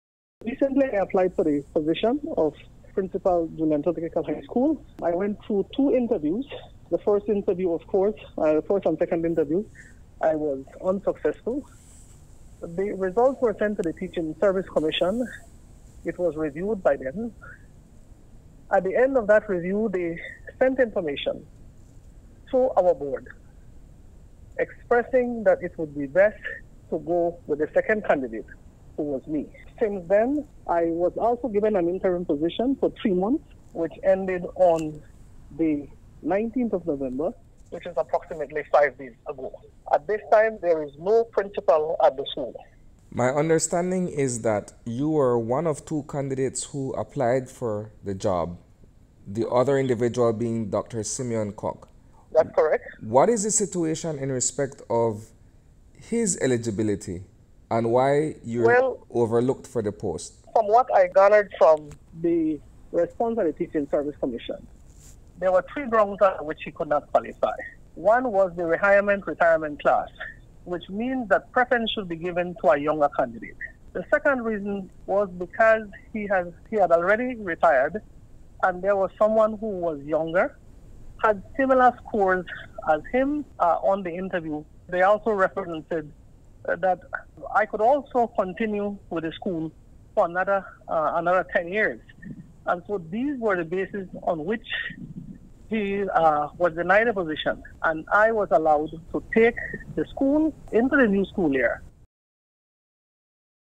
Julian Cho: Still No Principal at the Toledo High School
There are new developments at Julian Cho Technical High School in Toledo District where the board of directors met this evening at four o’clock to determine who will be the next principal of the secondary school.  Since November nineteenth, the institution has been without a principal since there is a deadlock on who will assume the vacancy.  News Five understands that there were two candidates vying for the post, including Doctor Simeon Coc and Doctor Kent Arzu.  Coc, who has reached retirement age and has stepped down, was deemed ineligible due to a clause in the terms of reference.  Doctor Arzu who has been at Julian Cho for the past seventeen years is next in line to become the principal but is not considered an option by the existing board.  He explains the deadlock.